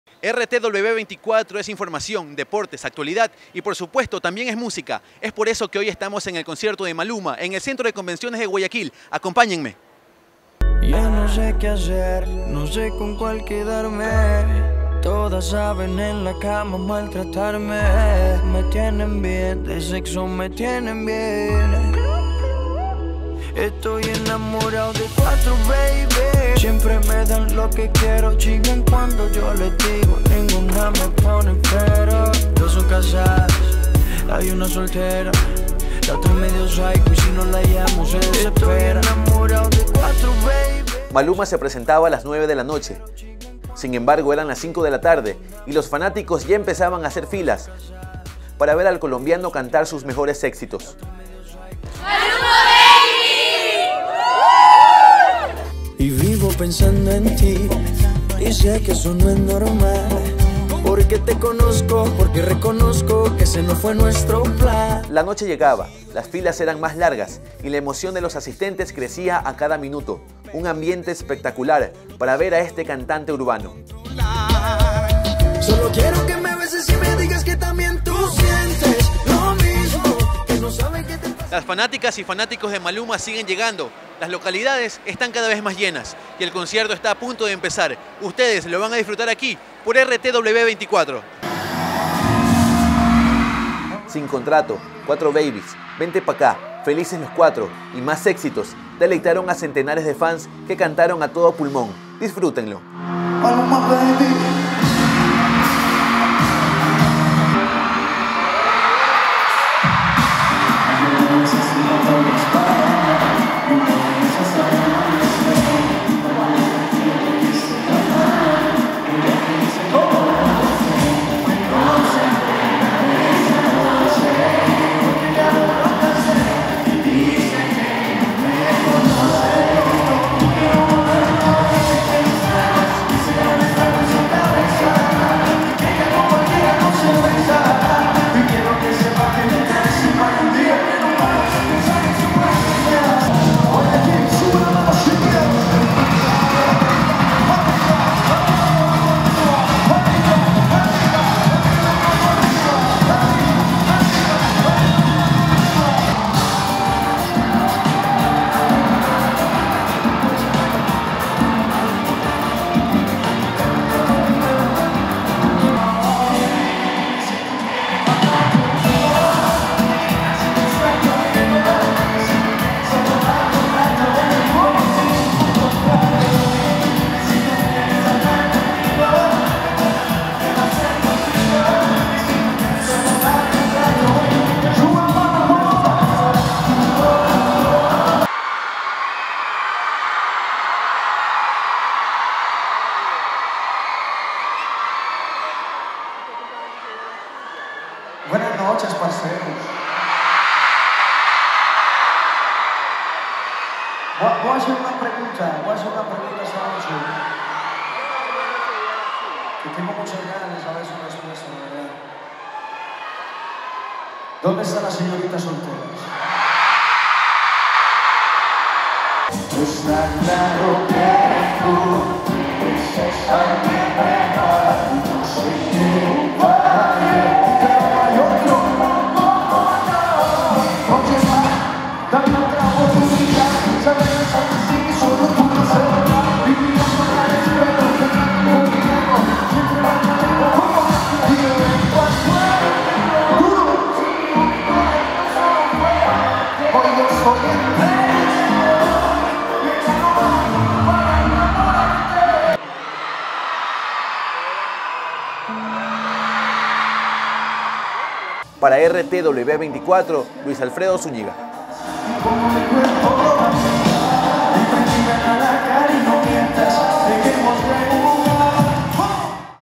ASÍ SE VIVIÓ I Concierto de Maluma en Guayaquil 2017
Conoce todo lo que sucedió antes y durante el concierto de Maluma en el centro de Convenciones de Guayaquil este 28 de noviembre con nuestro anchor Luis Alfredo Zúñiga por RTW24.